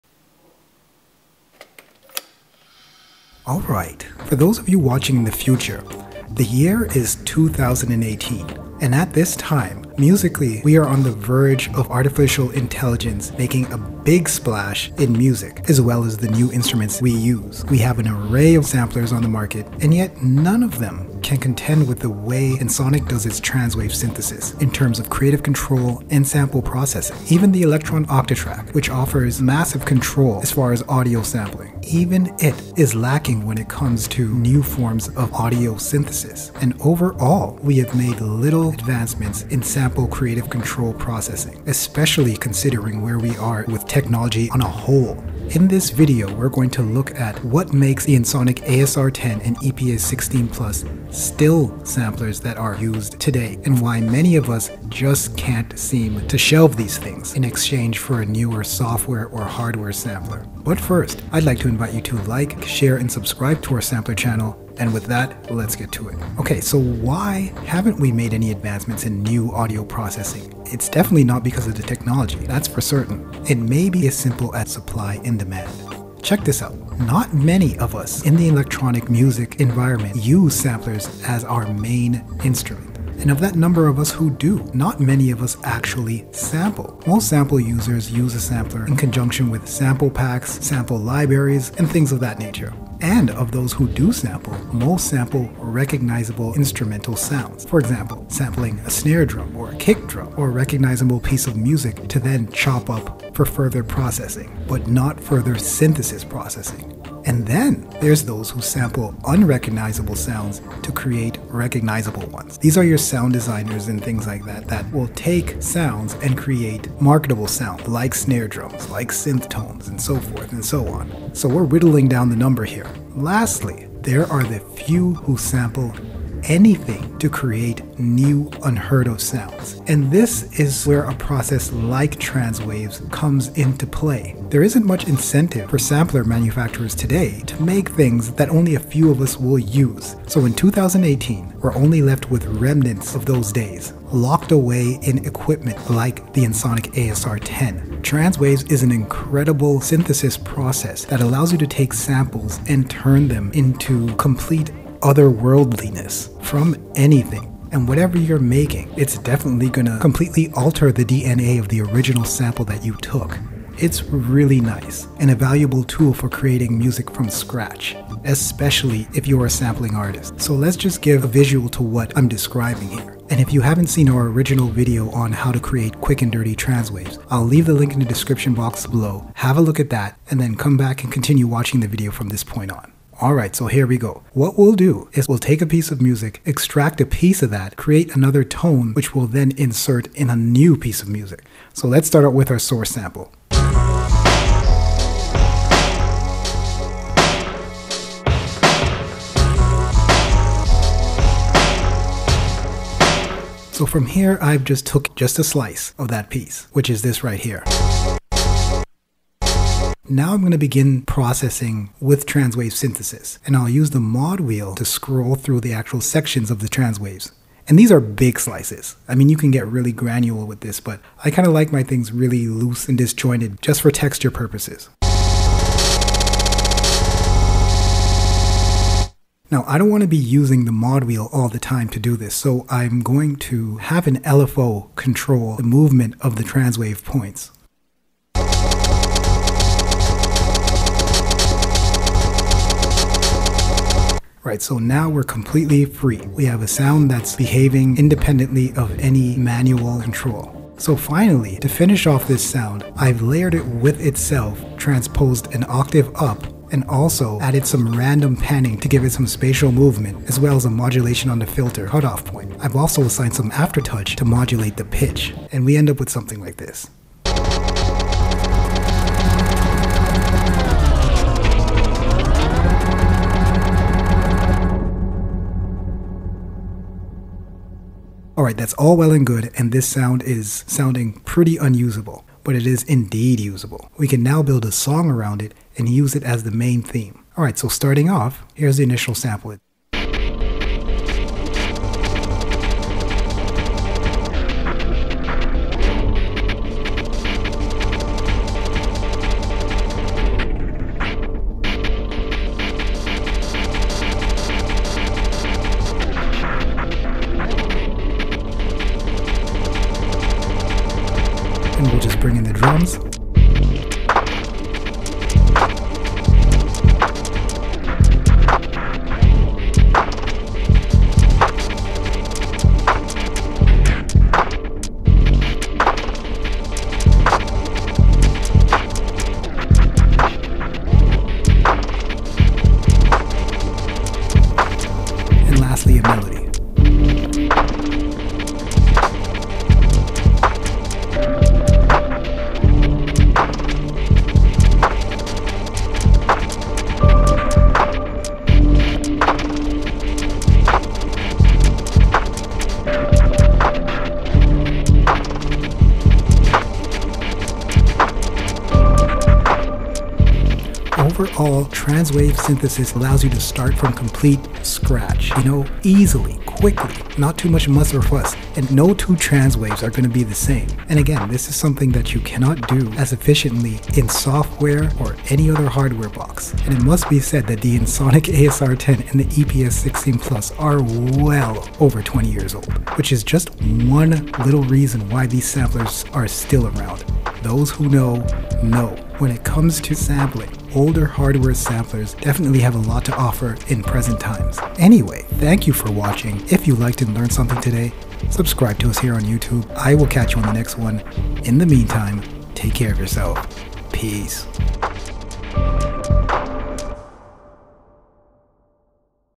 Why Samplers Don’t Have Technology Like Ensoniq Transwaves Synthesis Anymore
Why don’t we see any technology like Transwaves Synthesis on Samplers anymore? In this video we’ll explore a theory explaining why this might be. Using Transwave synthesis (a relative of wavetable synthesis) in a practical way can give great results and satisfaction. We’ll use the Ensoniq ASR-10 to demonstrate how Transwaves can be used in the beat making and song creation process.

Peace,

TDS